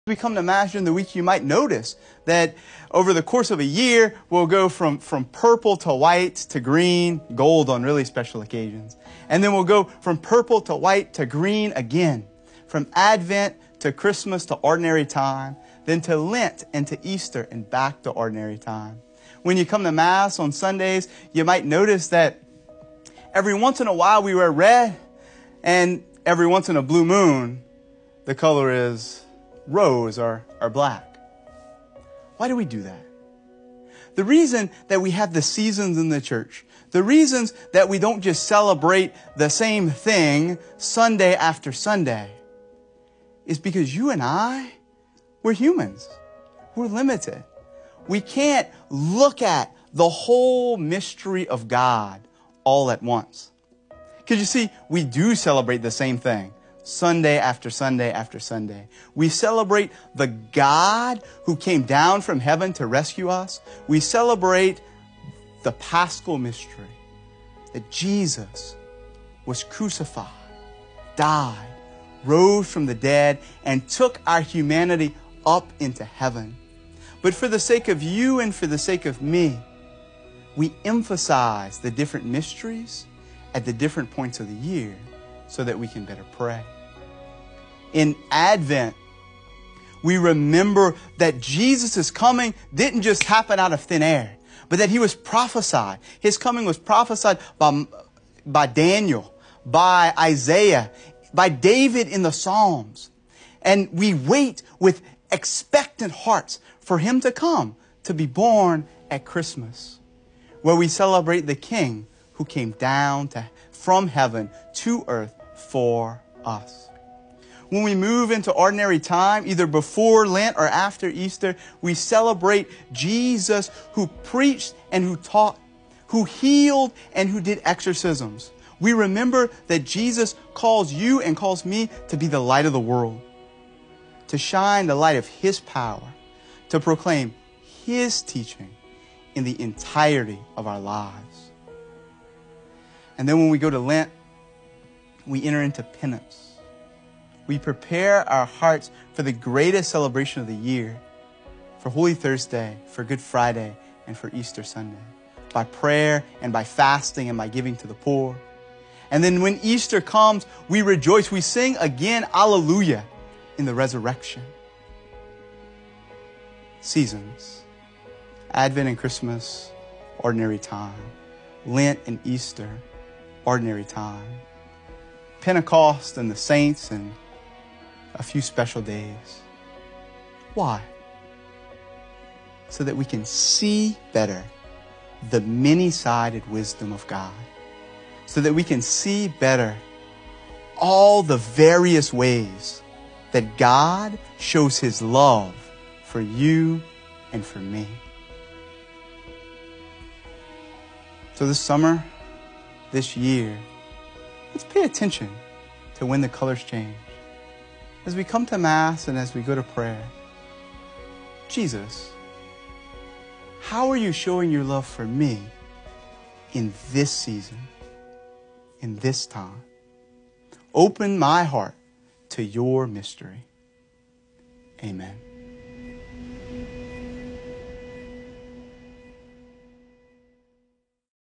Why Church Seasons?
Why do we have different seasons and different colors for each? Father Brice